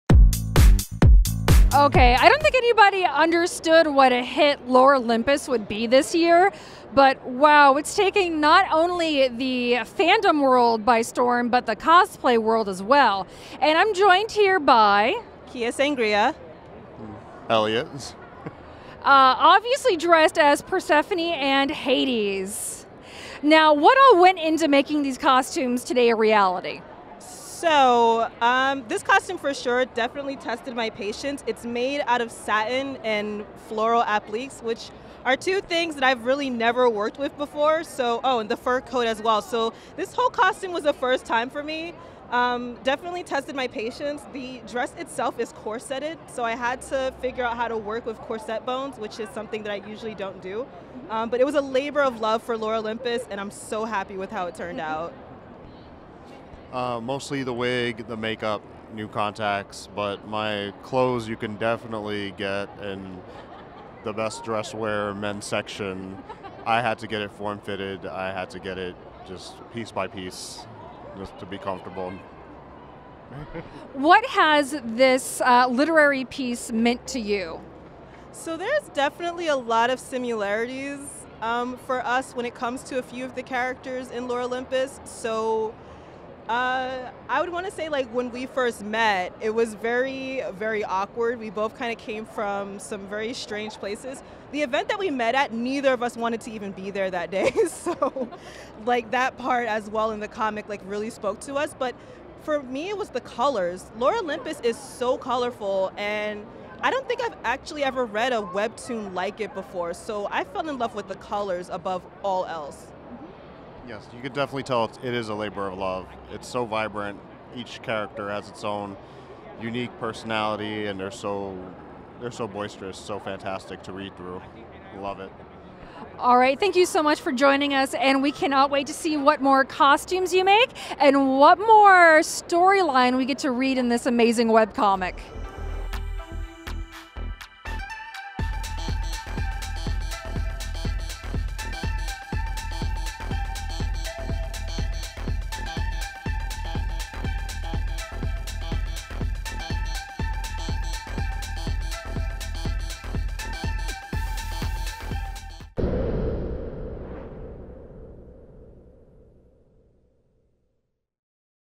Hades and Persephone! Lore Olympus Cosplay at Anime NYC 2019
At Anime NYC 2019, Lucky Grim interviews a pair of amazing Lore Olympus cosplayers.